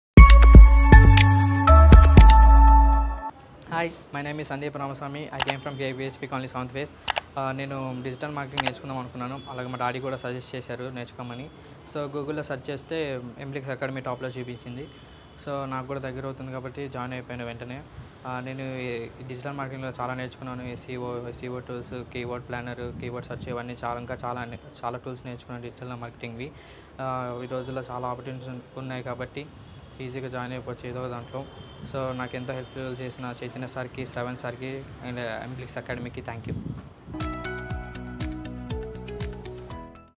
Digital Marketing Course Reviews | Students opinion on Digital Marketing Course Telugu | Emblix
If you're looking to start your career in digital marketing or enhance your existing skills, this video will give you a comprehensive look at the course structure, key learnings, and the feedback from real students. From SEO, Social Media Marketing, to Google Ads Certification, we'll cover all the essentials that make Emblix's digital marketing programs stand out.

Find out how you can benefit from this course, the success stories of past students, and whether Emblix is the right fit for you!